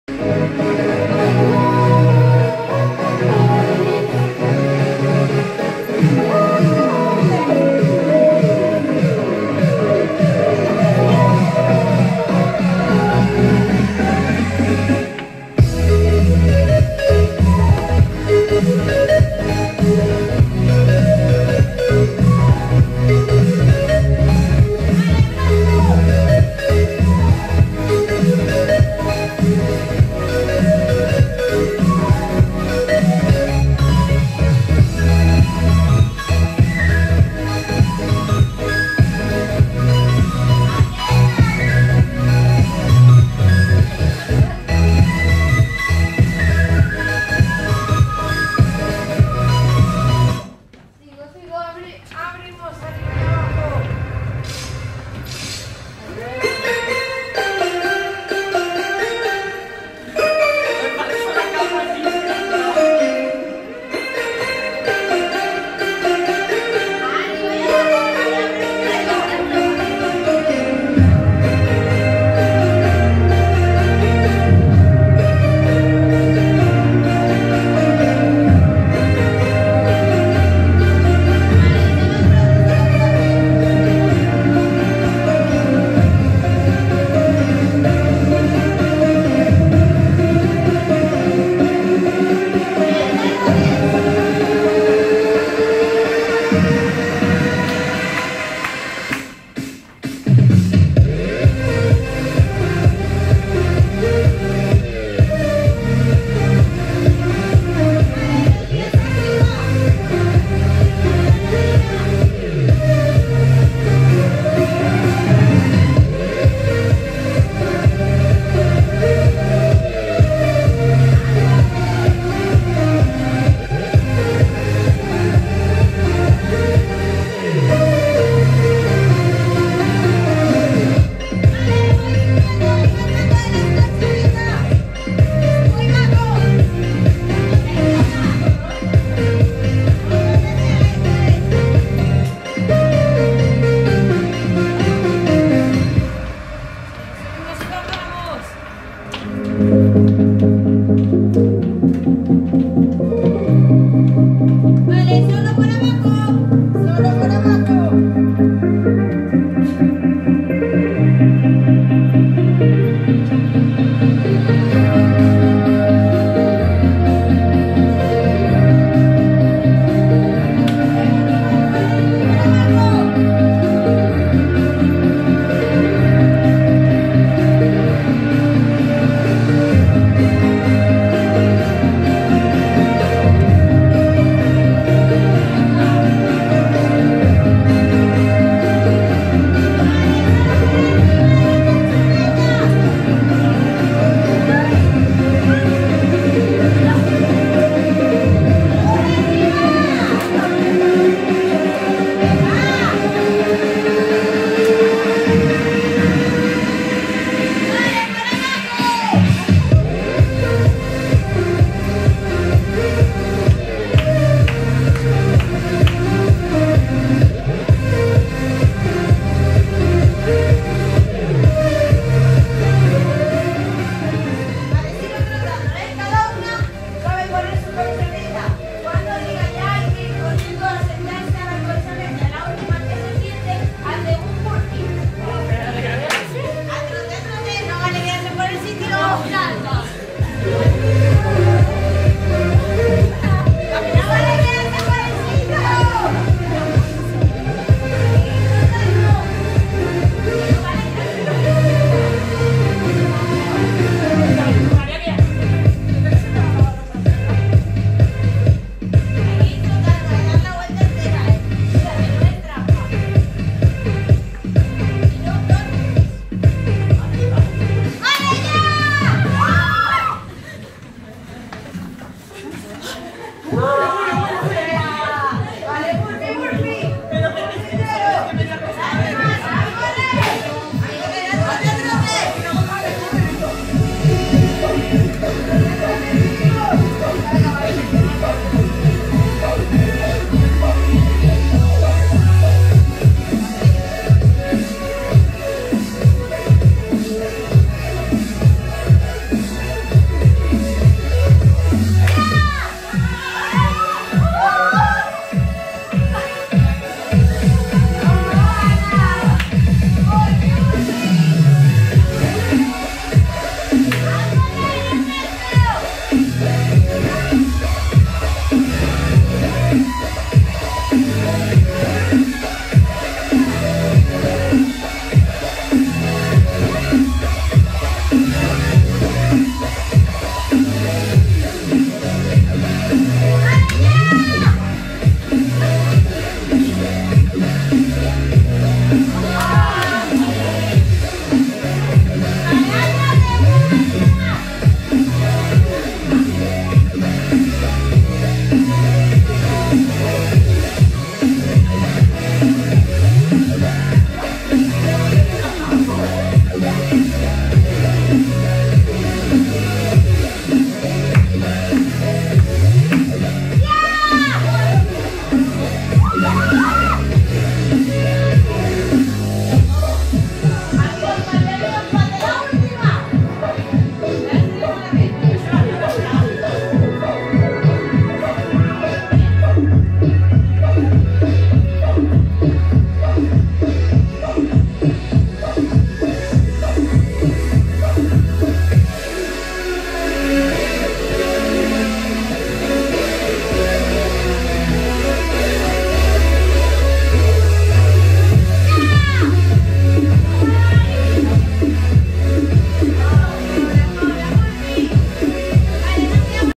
warm up 2 jumping fitness - entrenatt
warm up 2, otra forma diferente para tu calentamiento de las clases de jumping fitness.

Carrera con  movilidad articular,
Sube y baja de los trampolines
Juega con los trampolines, al sonido de la señal todas deberán ir a su trampolín, la última en sentarse deberá hacer un ejercicio físico.